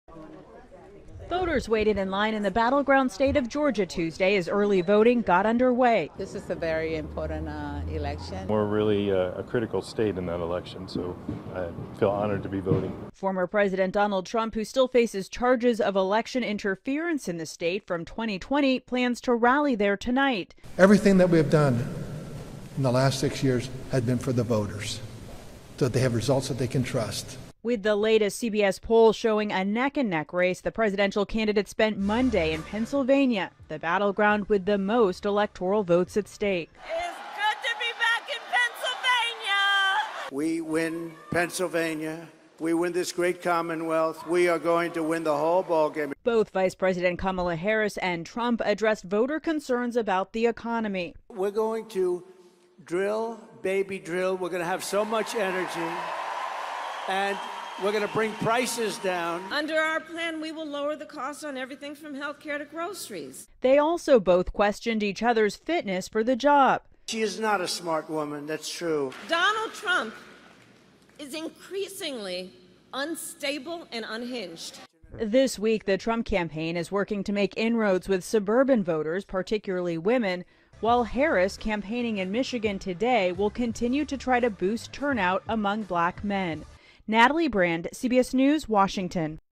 What are Kamala Harris and Donald Trump doing today?
Vice President Kamala Harris and former President Donald Trump have their eyes set on the all-important battleground states — Arizona, Georgia, Michigan, Nevada, North Carolina, Pennsylvania and Wisconsin.